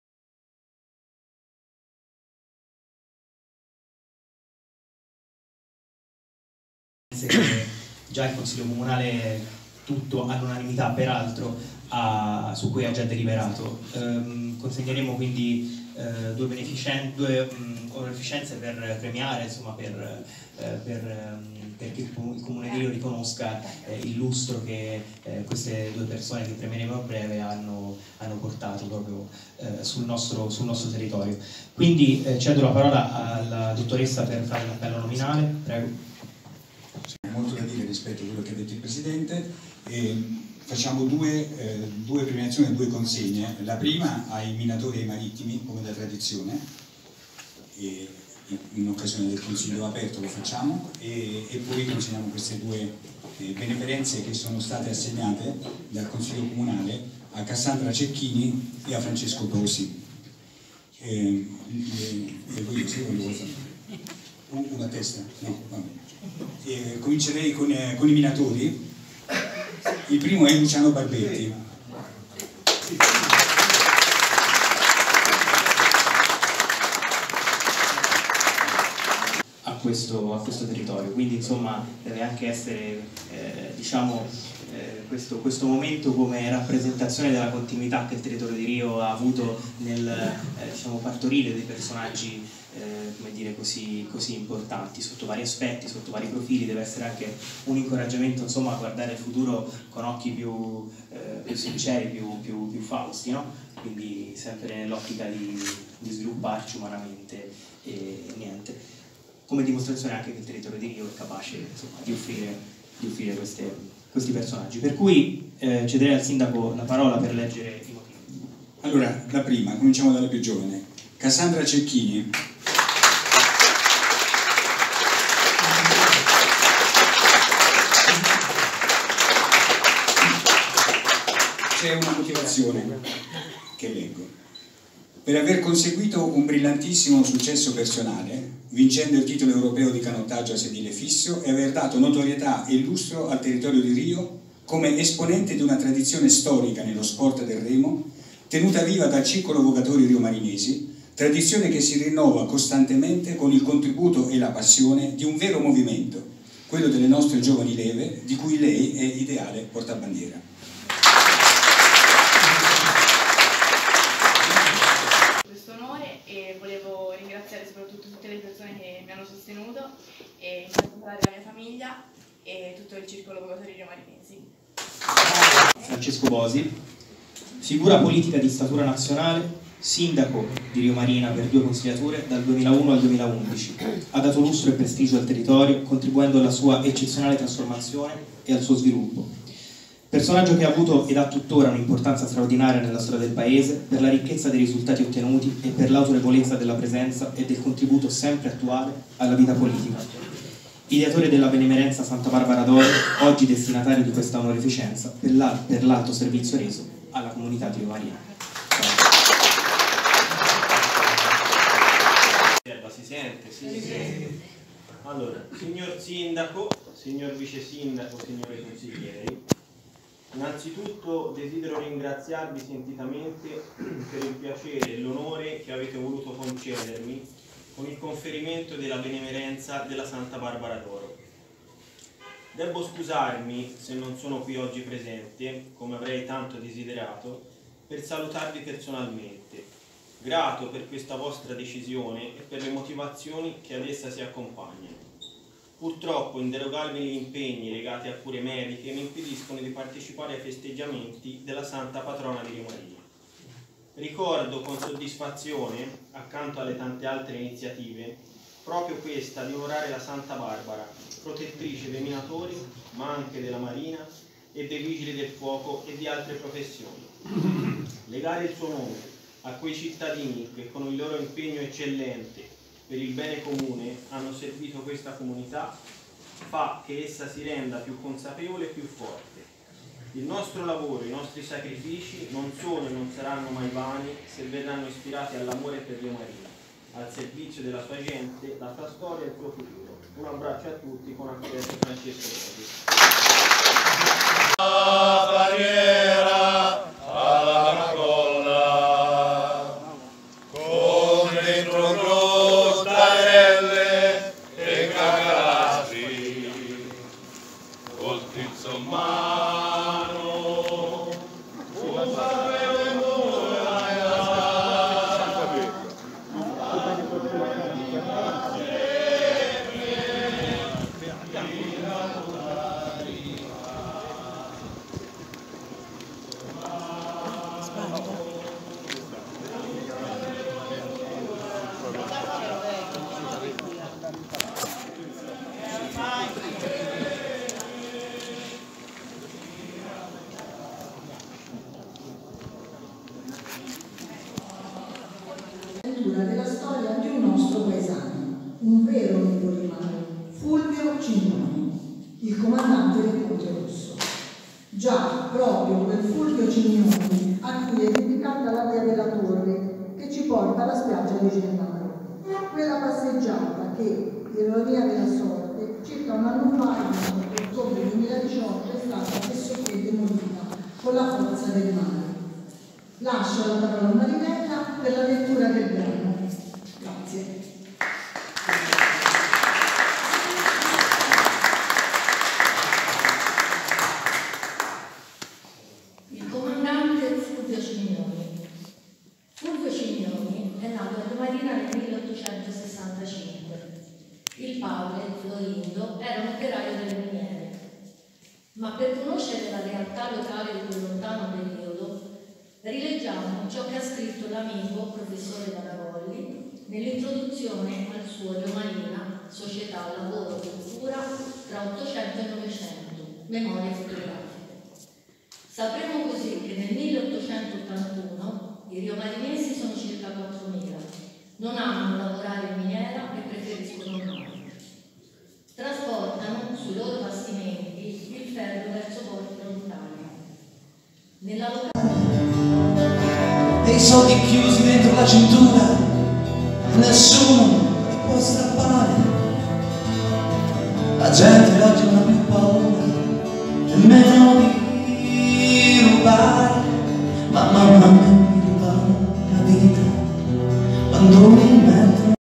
Rio Marina Festa di Santa Barbara 2019
Le celebrazioni della Festa di Santa Barbara a Rio Marina, nel Comune di Rio, con la consegna della targa omaggio ad ex minatori ed ex marinai e la consegna di due benemerenze civiche a Cassandra Cecchini e Francesco Bosi.